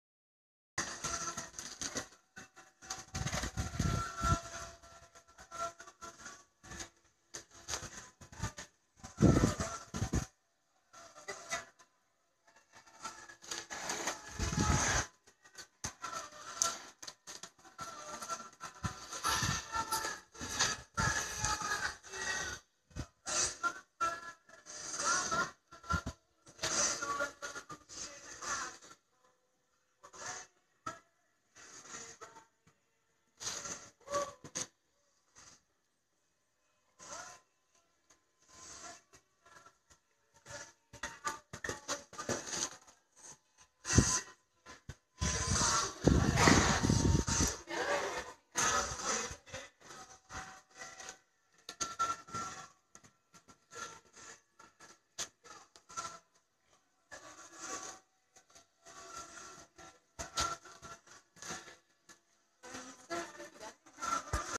Bareback riding, all gaits (1')
Riding Chistoso, our PRE Andalusian stallion, in all gaits bareback.